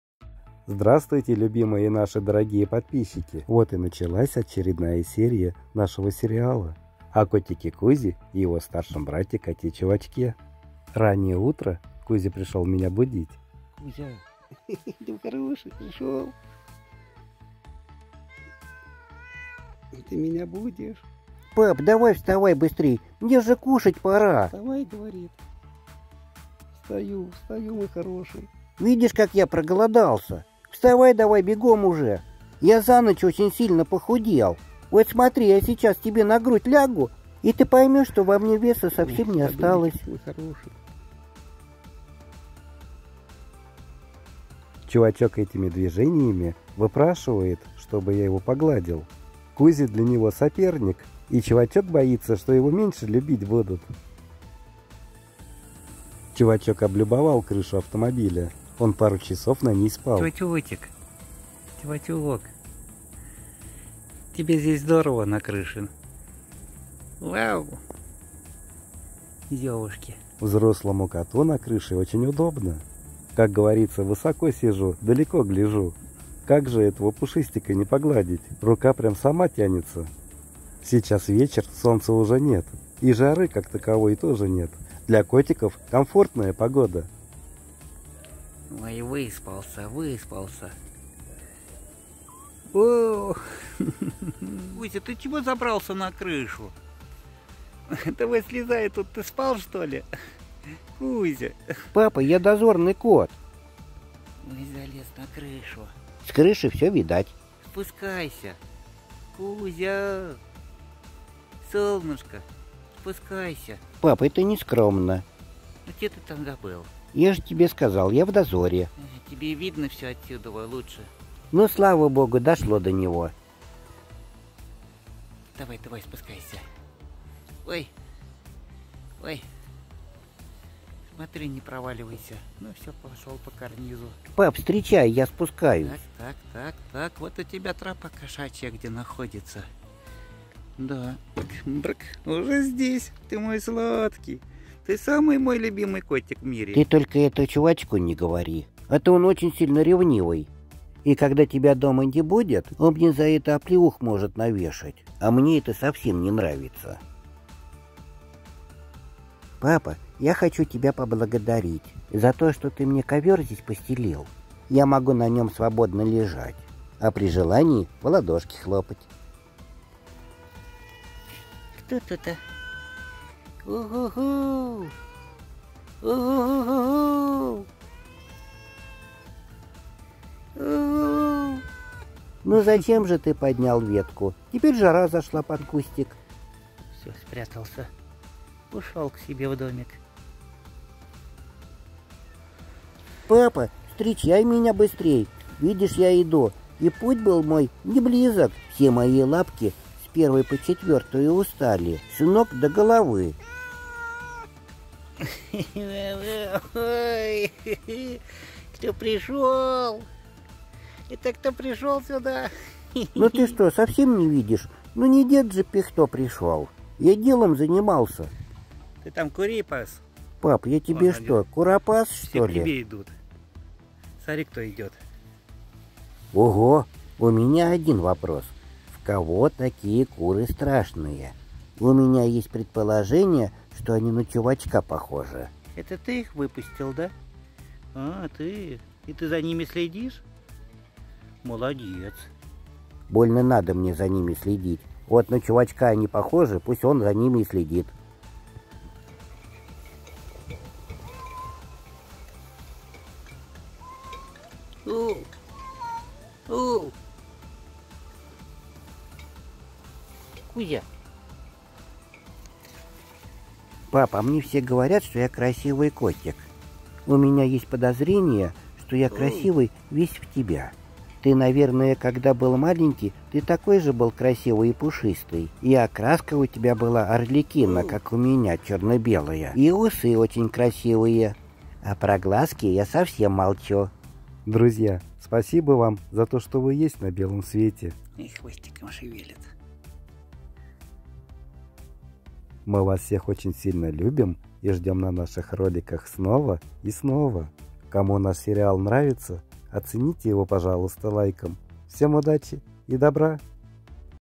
Дозорный котик Кузя. Кот не курапас. Коты Кузя и Чувачок. Говорящие коты.
В этом видео Вы увидите дозорного котика Кузю и его старшего брата кота Чувачка. Говорящие коты поднимут Вам настроение,  улыбка гарантирована!

Помощь от добрых людей на добрые дела:
Сбербанк
Игорь Анатольевич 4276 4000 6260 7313

постоянно новые видео!

3.
4.
5.